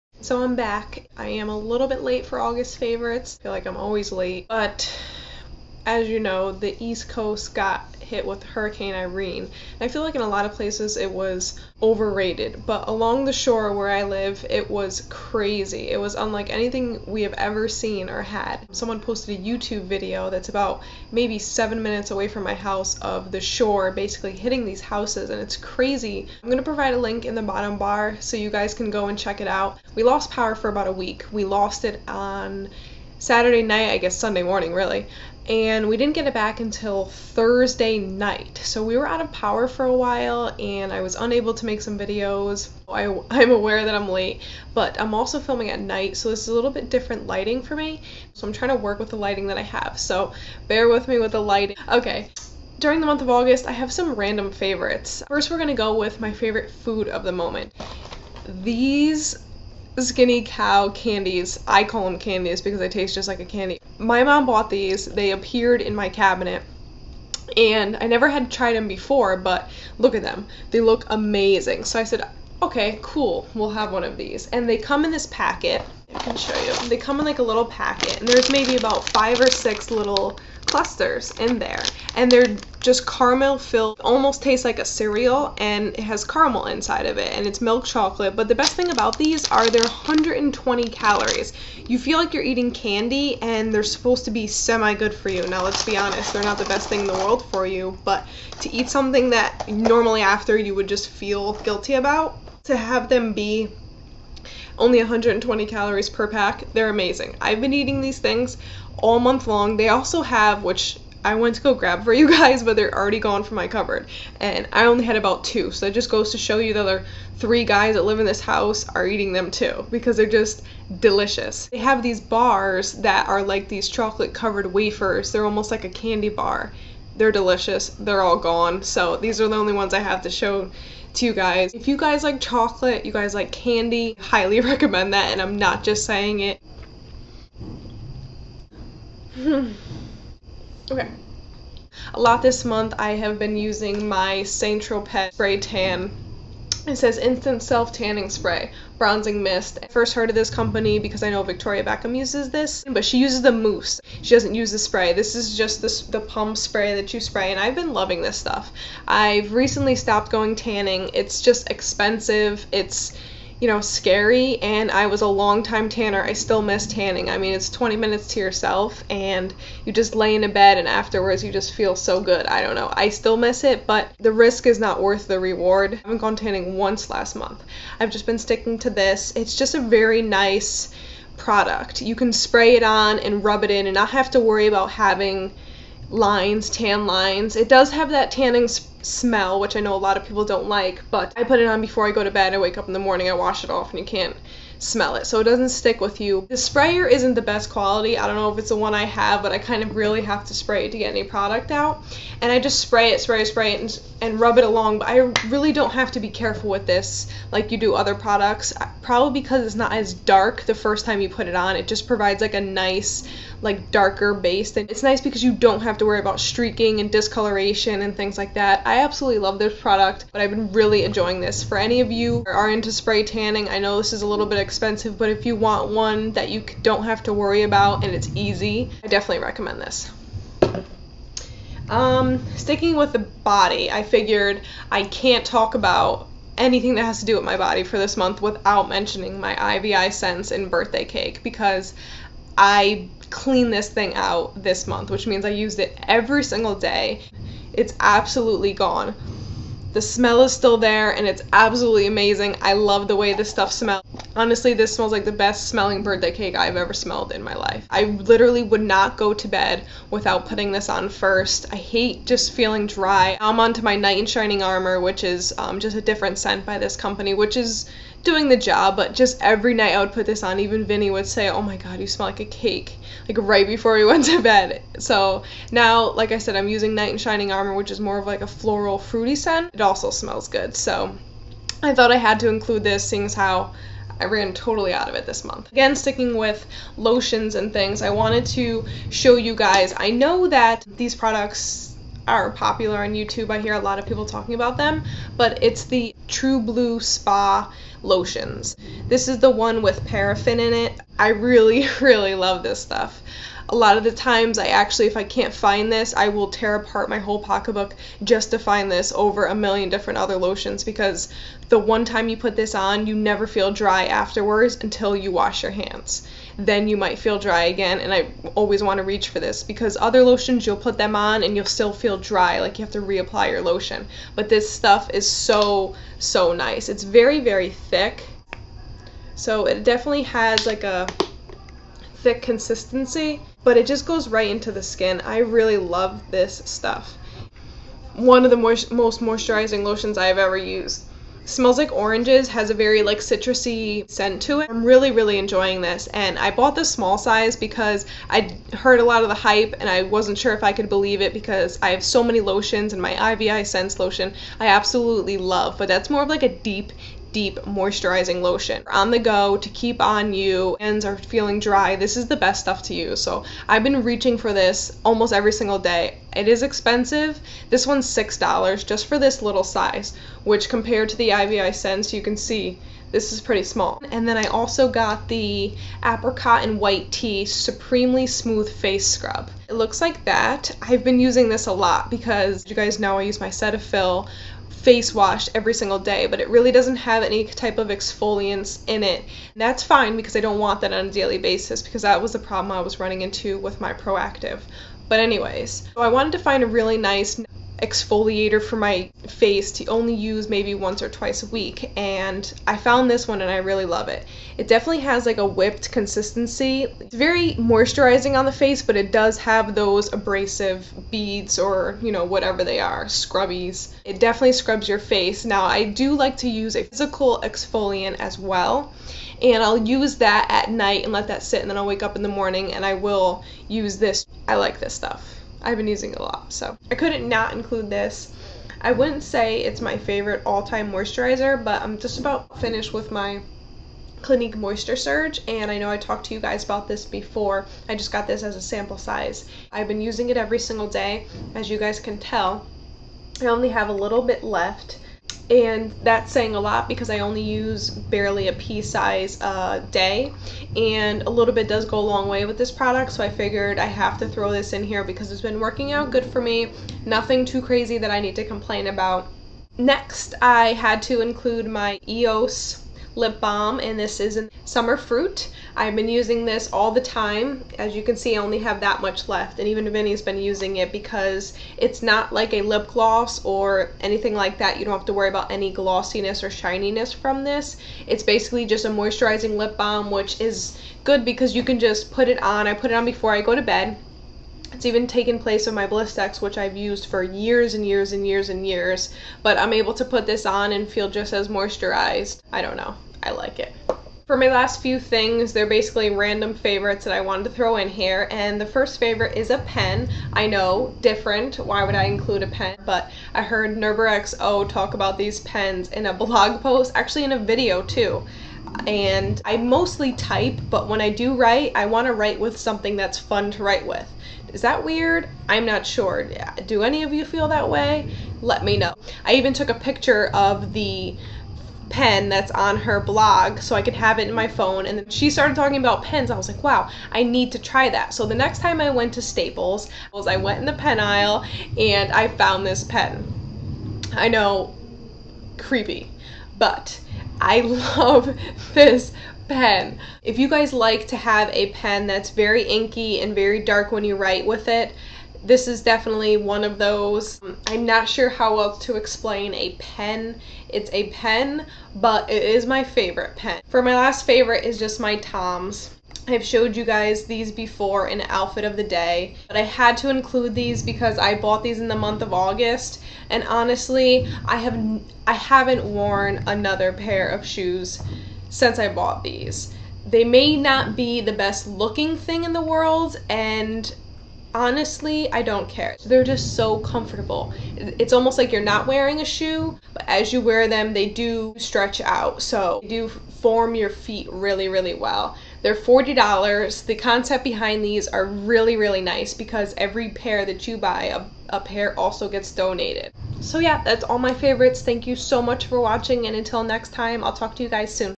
August 2011 Favorites!
Mentioned:
Skinny Cow Milk Chocolate Clusters *Dark Choc are delish too! :)
iviscents Body Frosting in Birthday Cake
iviscents Body Frosting in Knight in Shinning Armour
True Blue Spa Lotion w/ Parafin
True Blue Spa Face Scrub w/ Apricots & Shea
EOS Lip Balm mine is in Summer Fruit
St. Tropez Bronzing Spray
Bic Pen 537R
Toms Classic Shoe in Black
Clinique Moisture Surge